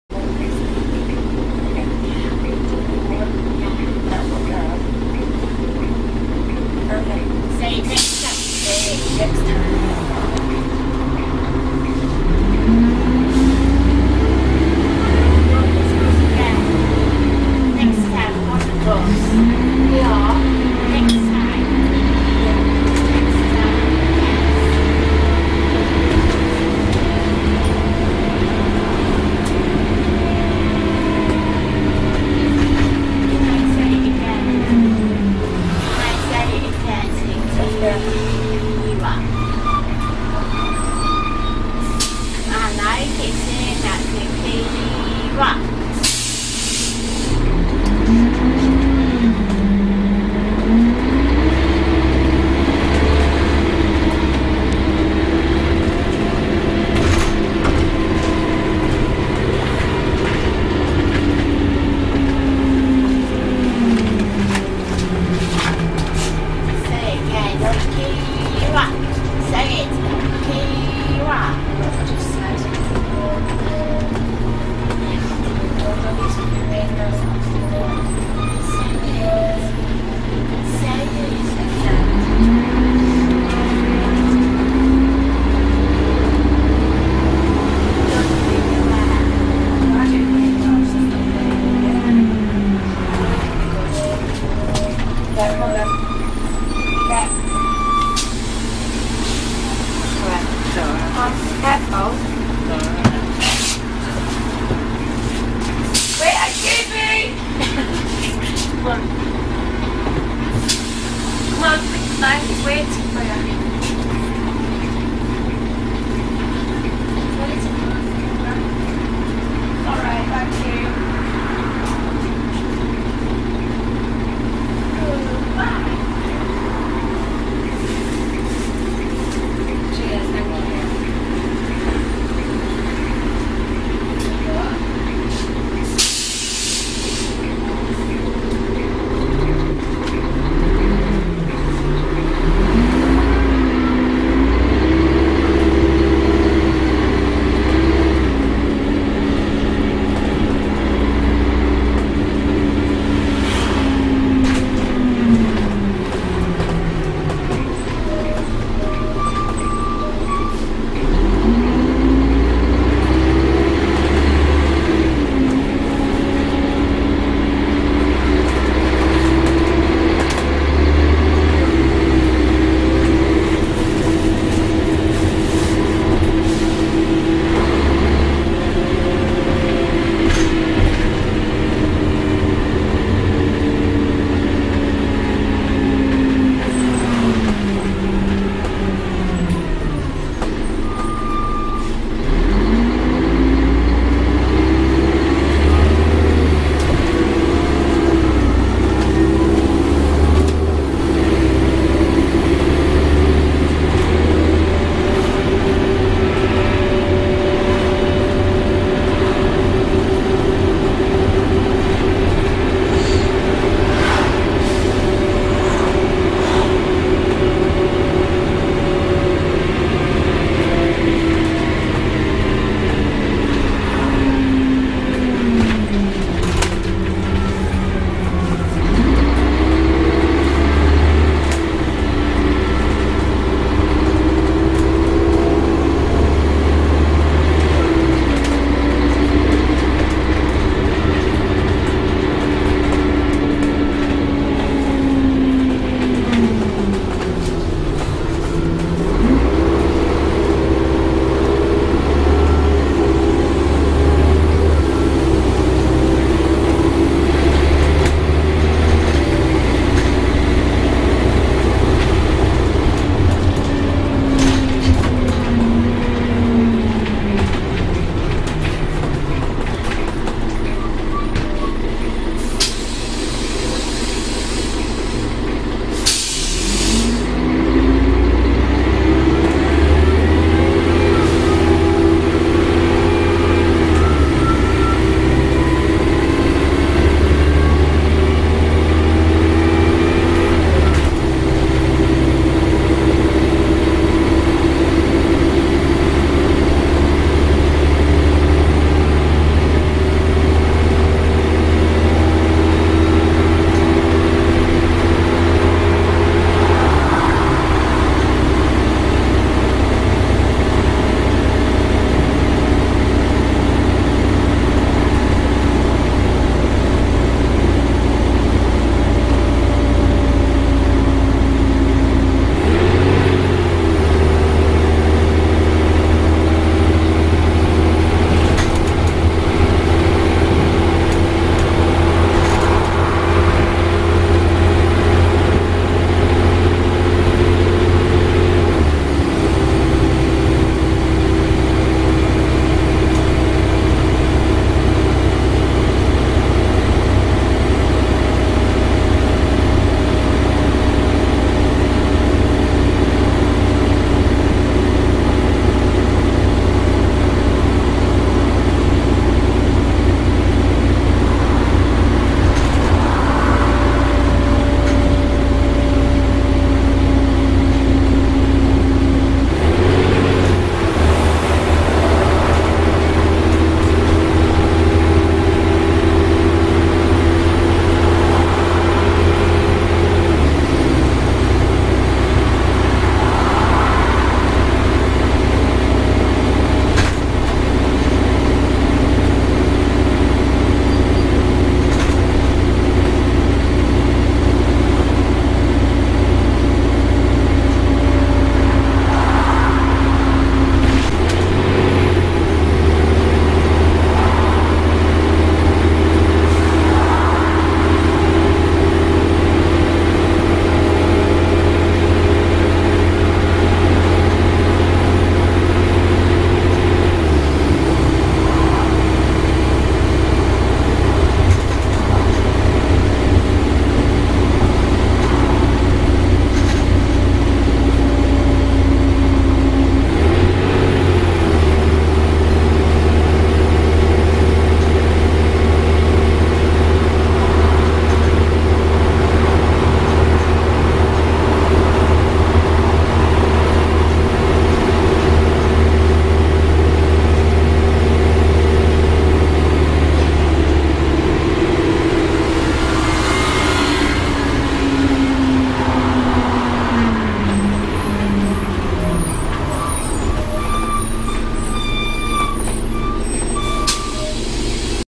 First WY Alexander Royale 30792 (R612 JUB)
Taken on 30792 (R612 JUB) from Halifax to Huddersfield on service 503.

This was filmed on 31/10/2011.